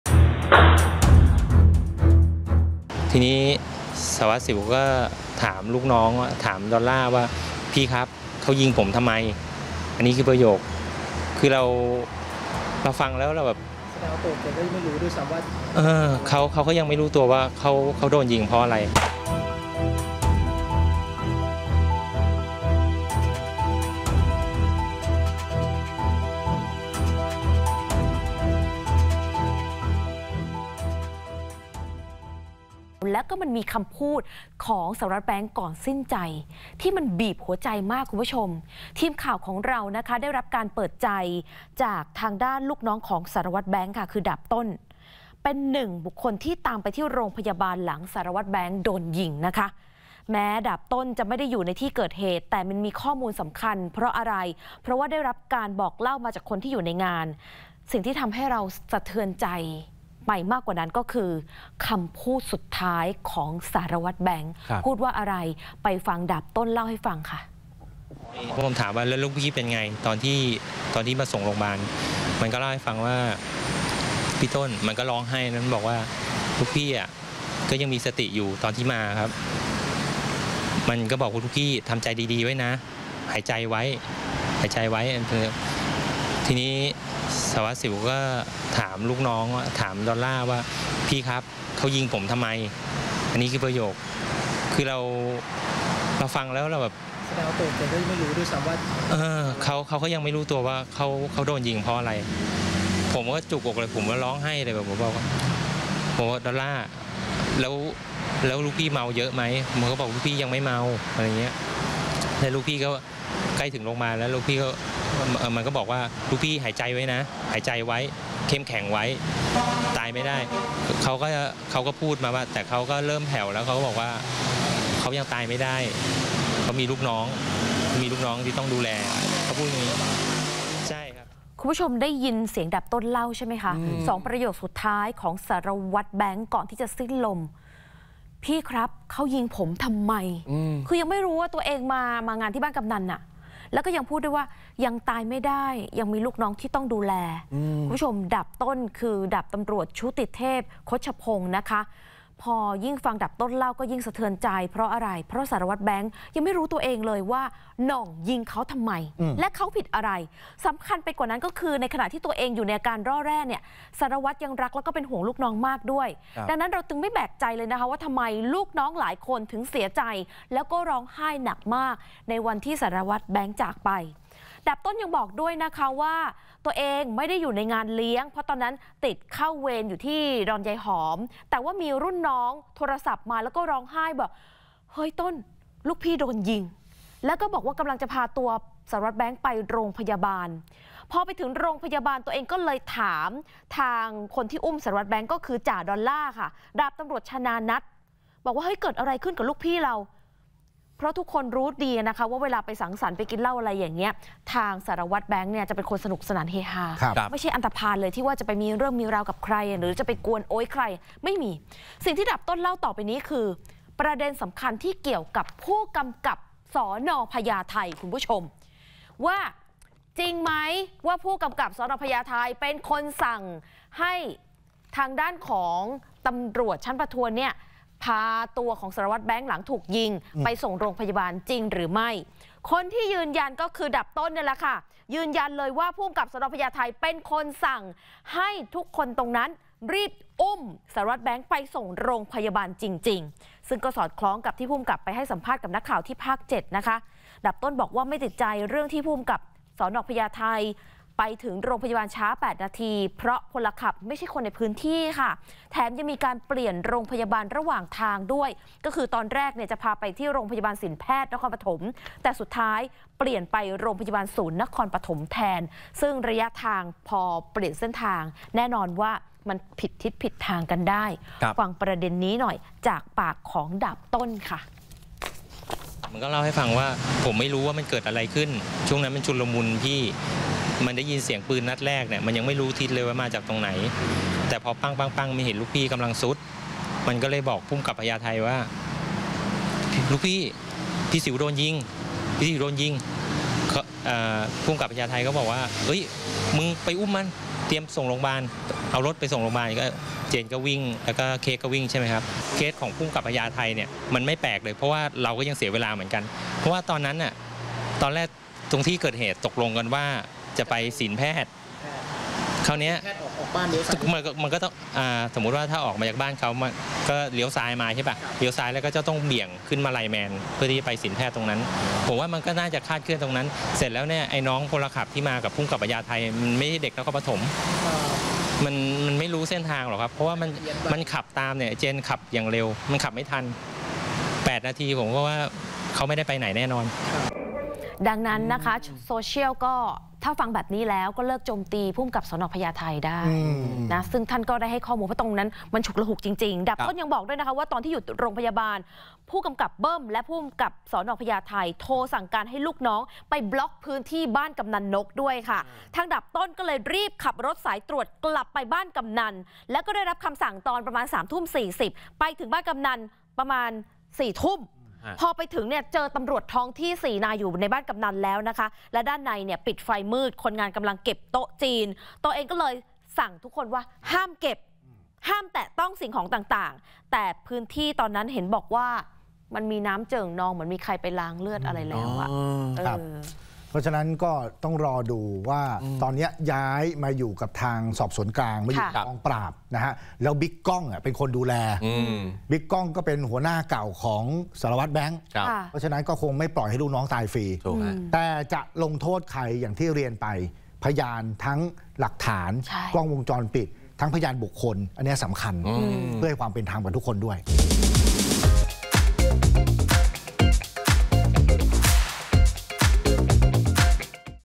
สุดบีบหัวใจ เปิดคำพูดสุดท้าย ของสารวัตรแบงค์ | ข่าวใหญ่ ออนไลน์ | ข่าวช่อง8
ลูกน้องเล่าวินาทีสุดท้ายของสารวัตรแบงค์ พร้อมคำพูดสุดสะเทือนใจ และยืนยันว่า ผกก.พญาไท ให้การช่วยเหลือ “สารวัตรแบงค์” จริง